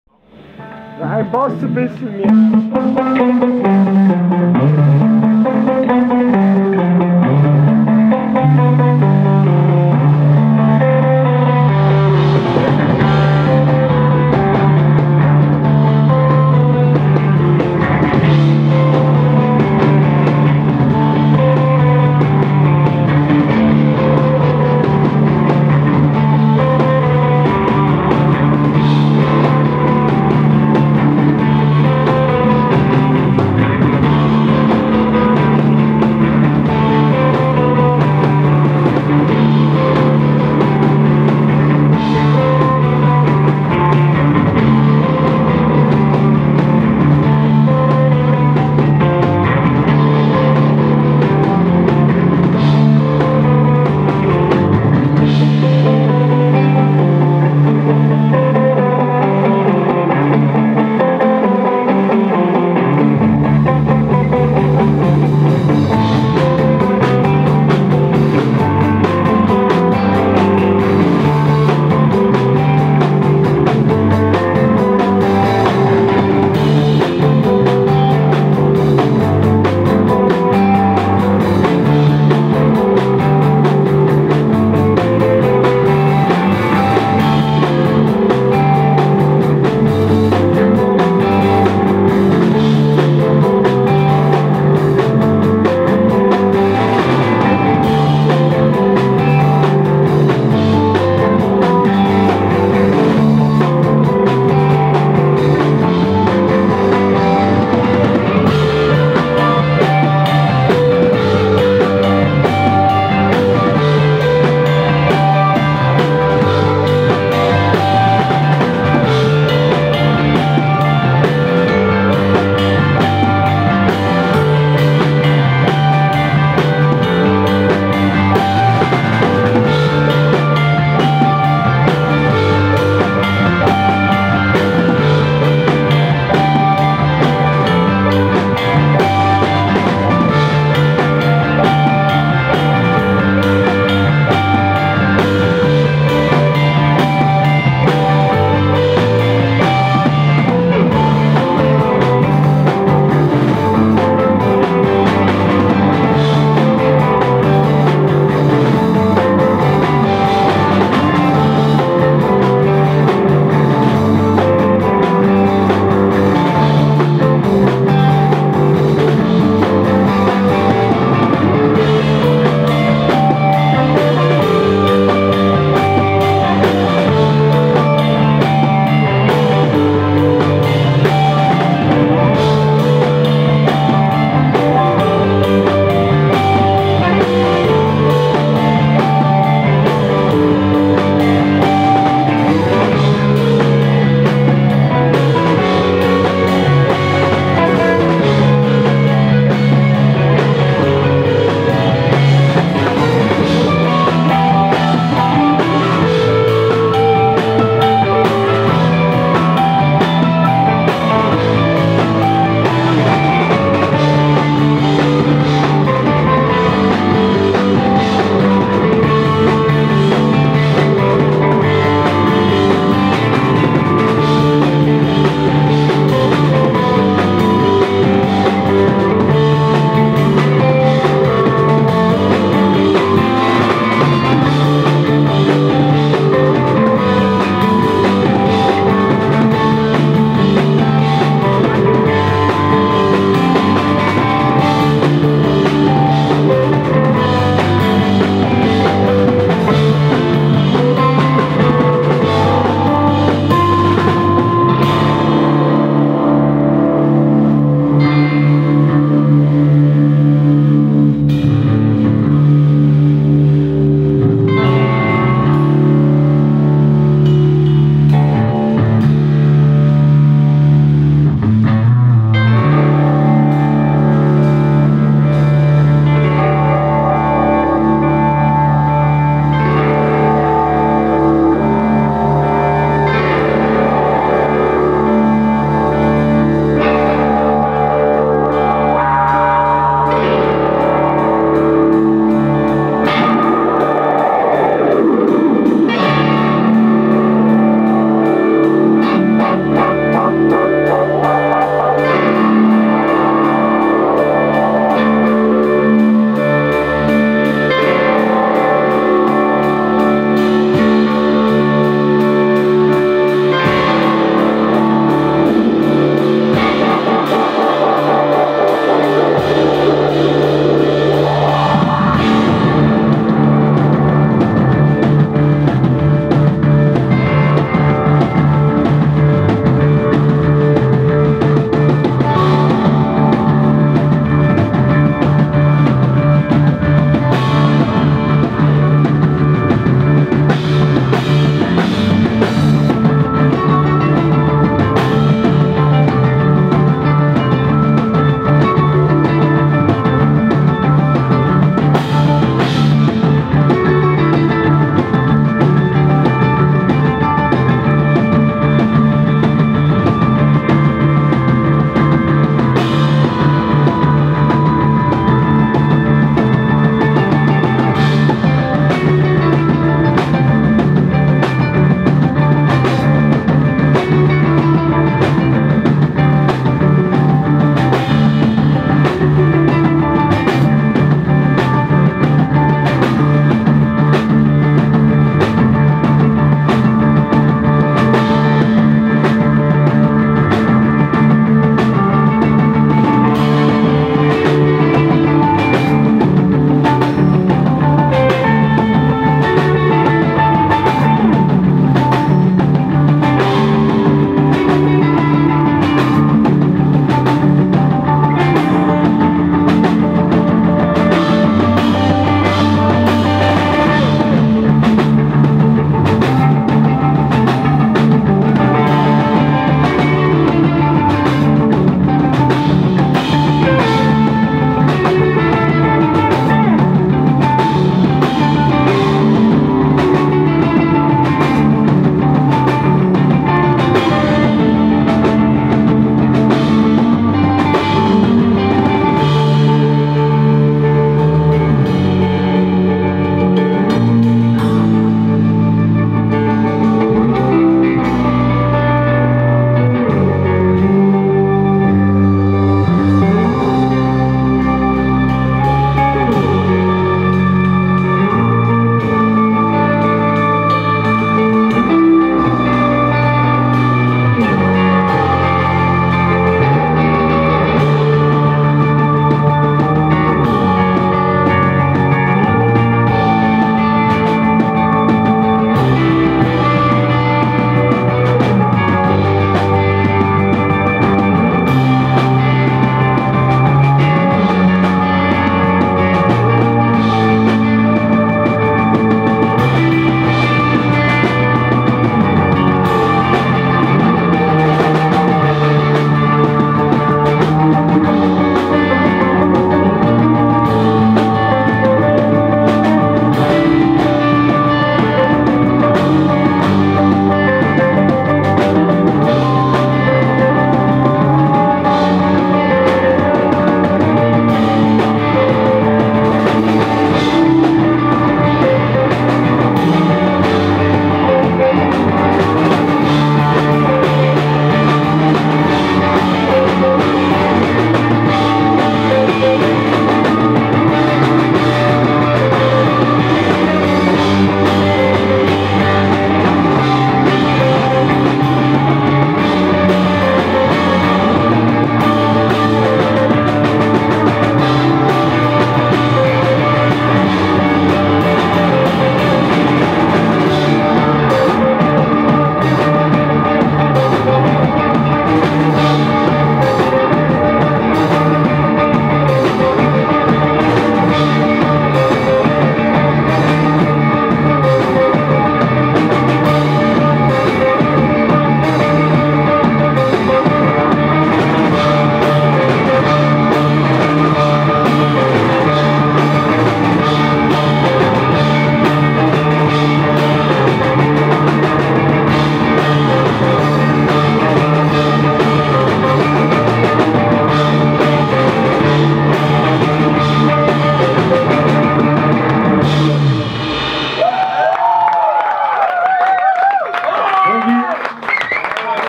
Sonson live, W1 Regensburg 130302
Sonson from Göteborg, SWE on live in Regensburg, DE. 2013 at W1.

Good folks, great times!